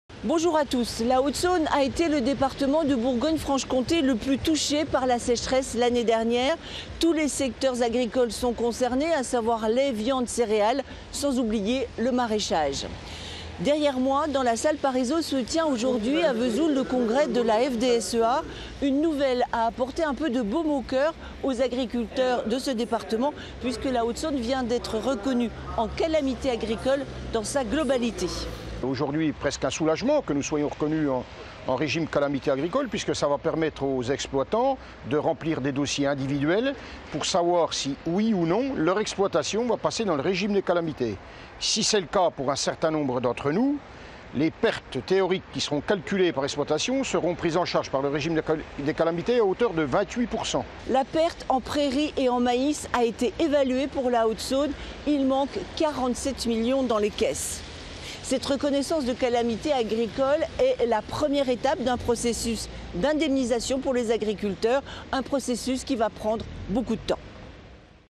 Sécheresse 2018 : la Haute-Saône reconnue au titre des calamités agricoles
Avec Thierry Chalmin
Président de la chambre d'agriculture
de Haute-Saône
reportage C.Eme-Ziri, D.Colle, G.Marlier